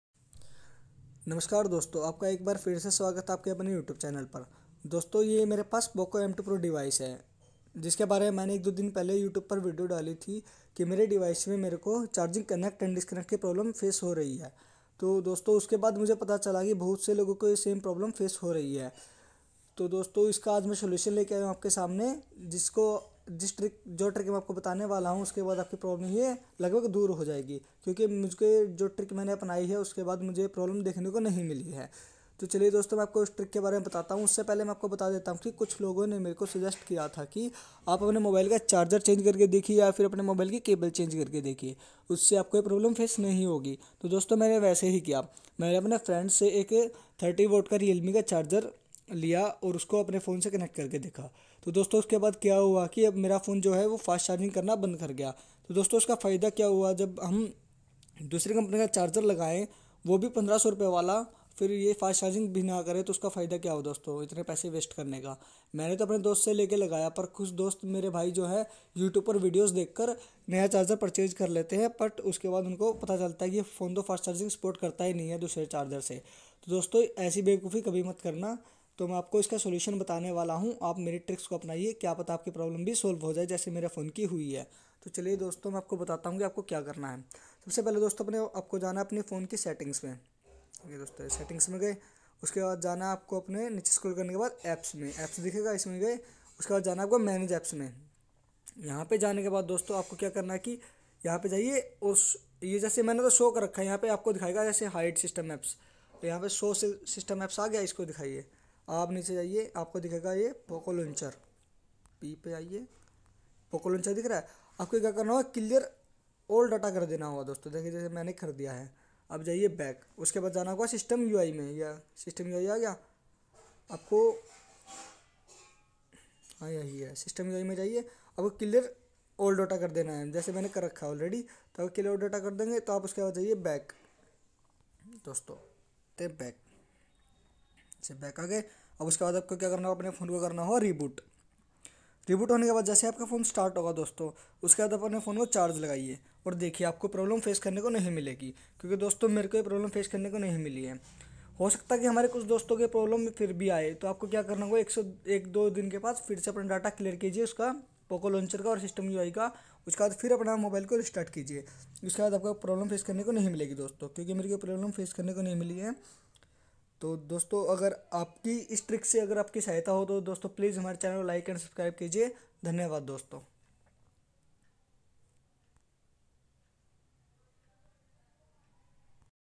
Poco M2 Pro charging problem & issue solution🔥🔥🔥🔥 tips and tricks
Is video me main aapko Poco M2 Pro mein aane wali charging problem ka solution bataunga ki  kis trah aap apne mobile ko thik kar sakte ho mere dwara batai gyi tips and tricks ke dwara.